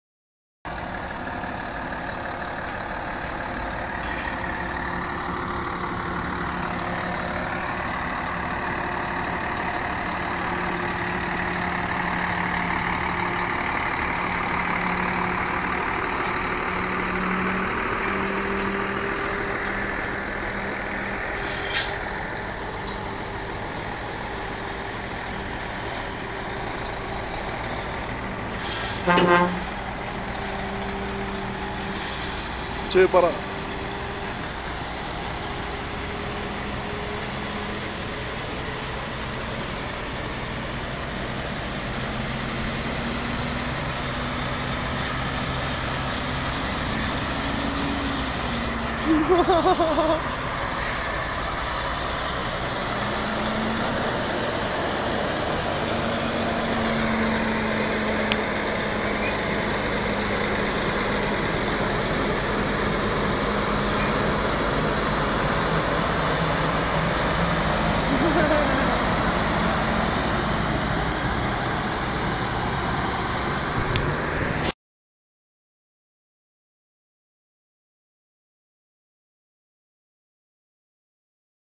Fielding field days '13
Truckn out the farm gear at manfield